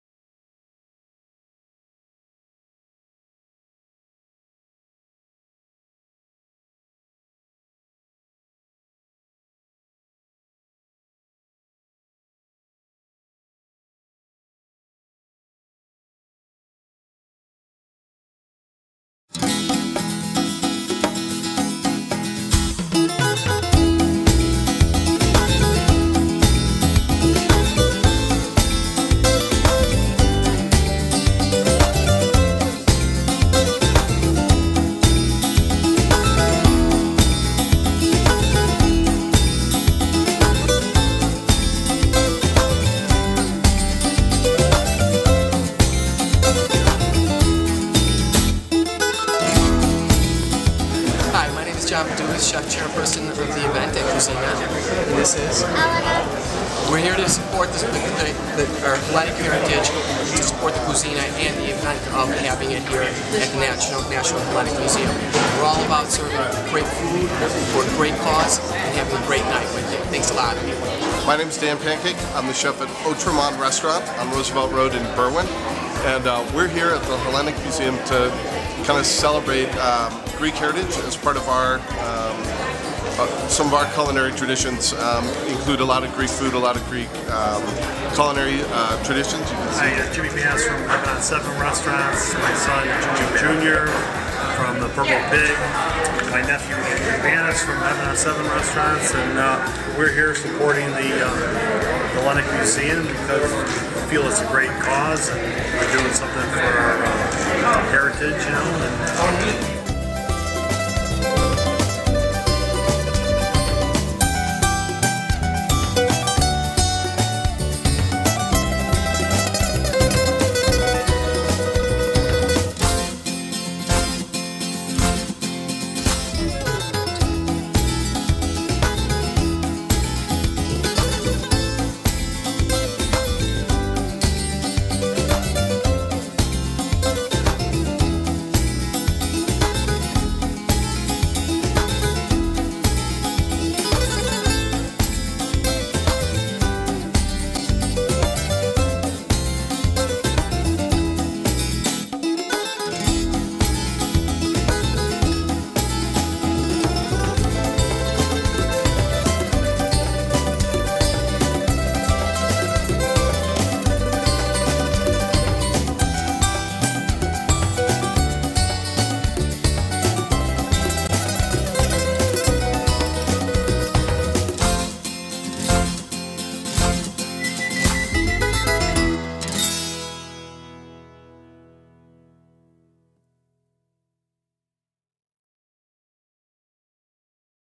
Kouzina 2012
2012 Kouzina was on September 20, 2012. Taste the food prepared by top chefs from around Chicago who will whip up Greek and Mediterranean delectable bites

Taste the wine enjoy premium wine tastings from Greece

Taste the culture sample and shop at the pop up "Agora" (The Marketplace) featuring authentic Greek foods and products